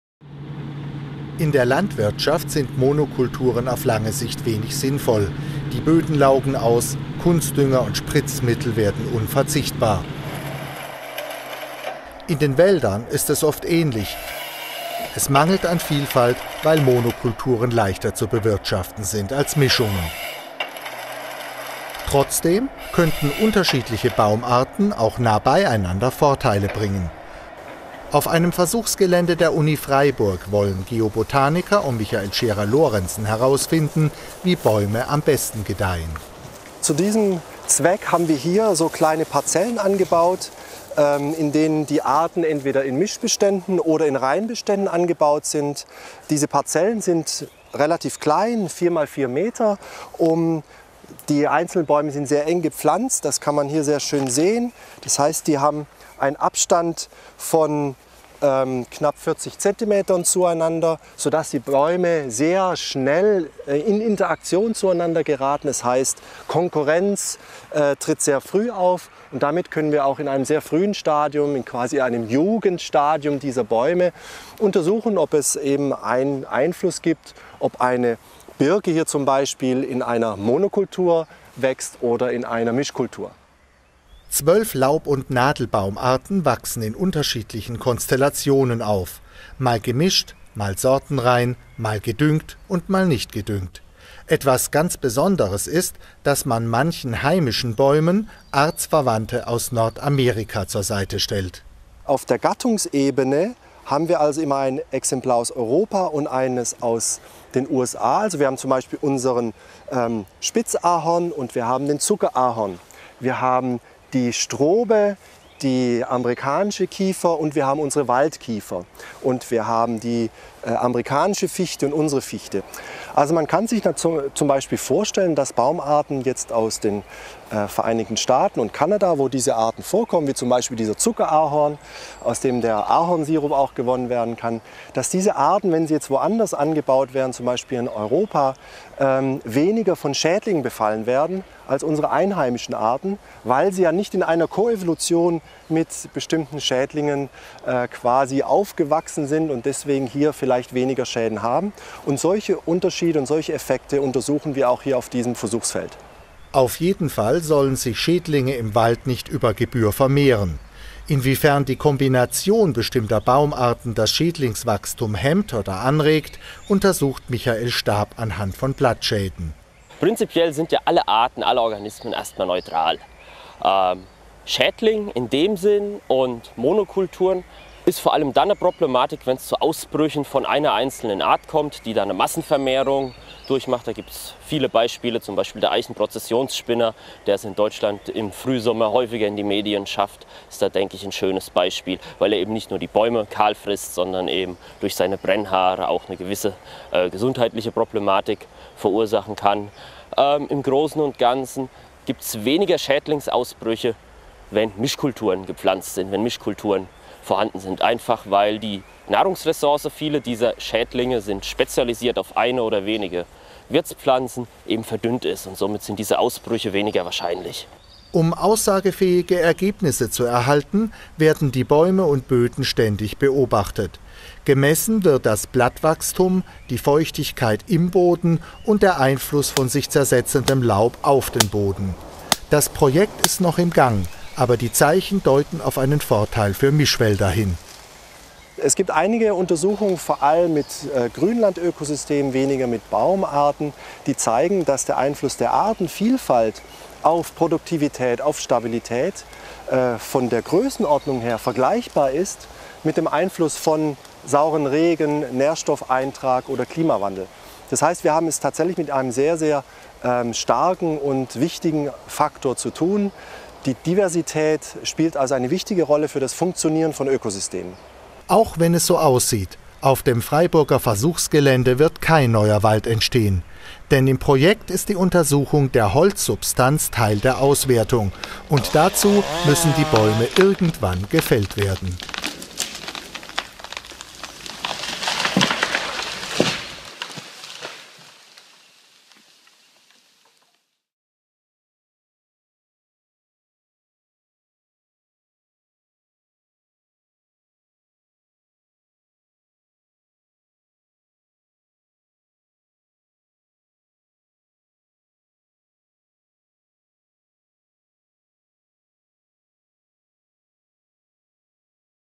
Michael Scherer-Lorenzen - Baumwachstum in Mono- und Mischkulturen - Uni Freiburg
"Gleich und gleich gesellt sich gern." In Paarbeziehungen bei Menschen hat sich dieser alte Spruch nicht bewahrheitet. Ob der Satz für Bäume gilt, untersuchen Geobotaniker der Uni Freiburg. In einer eigens dafür eingerichteten Pflanzung vergleichen sie in Monokulturen wachsende Bäume mit Vertretern derselben Art in Mischkulturen.